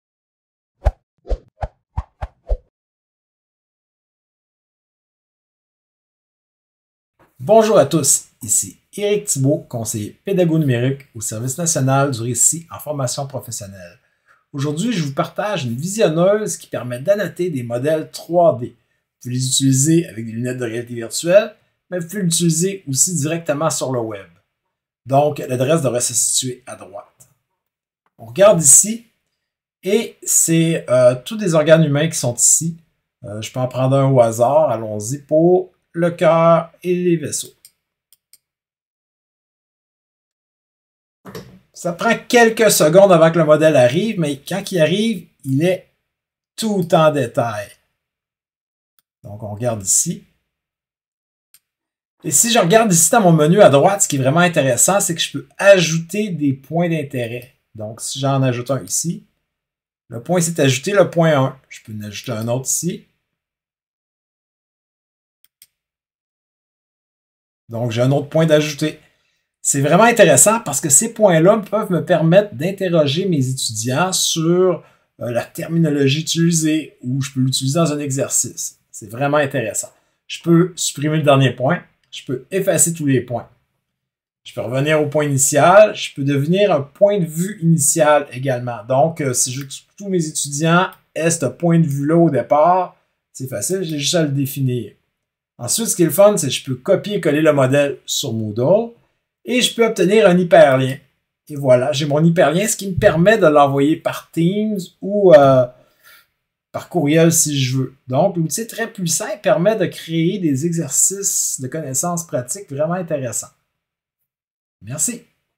Présentation de l'outil VAMO3D
Présentation de l'outil de visionnement d'organe humain à l'aide de la réalité augmentée.